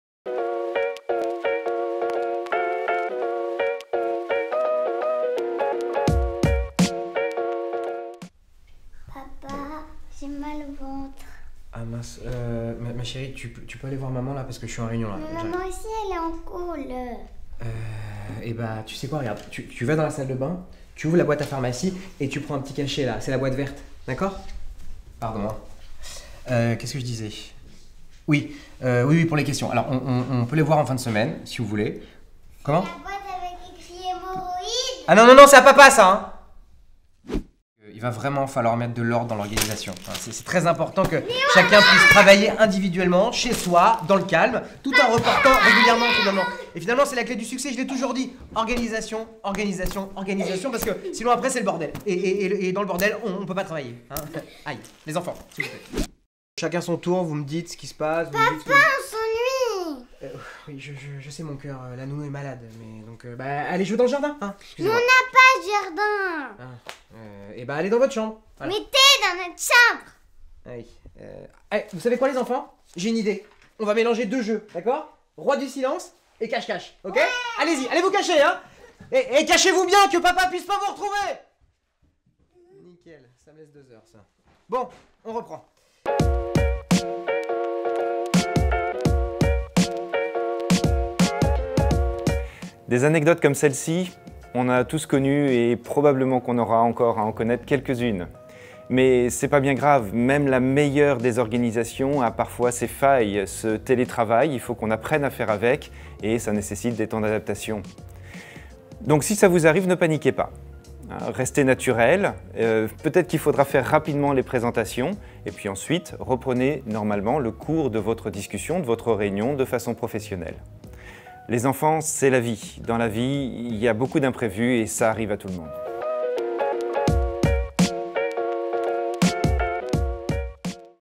EP04 : Télétravailler avec ses enfants
Préparez-vous pour la nouvelle websérie qui va faire trembler Netflix 😉

Au programme : des scènes loufoques et drôles agrémentées de conseils pour une vision décomplexée du télétravail chez Mazars !
La culture managériale bienveillante, la liberté de garder la main sur son équilibre vie pro/vie perso, l’esprit d’équipe chez Mazars… ça vous parle ? 😊